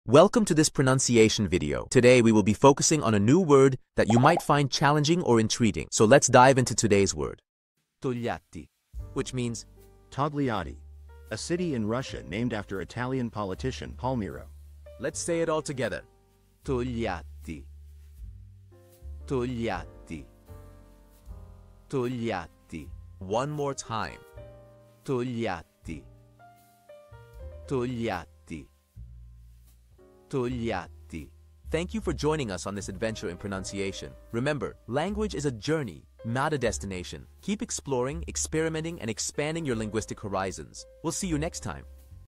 How to pronounce Togliatti
Master the Pronunciation of 'Togliatti Which Means Togliatti' - which means : Togliatti: A City in Russia Named After Italian Politician Palmiro. 📝 with @PizzaPronunciation 🎓🗣️ - [Your Guide to Italian]

Learn to pronounce 'Togliatti' perfectly in Italian with our easy and engaging tutorial by @PizzaPronunciation! 📖📽️ In this comprehensive video, we provide you with native speaker examples 🗣️👂 and phonetic breakdowns 🎵 to ensure you get the pronunciation just right.

How to say Togliatti in Italian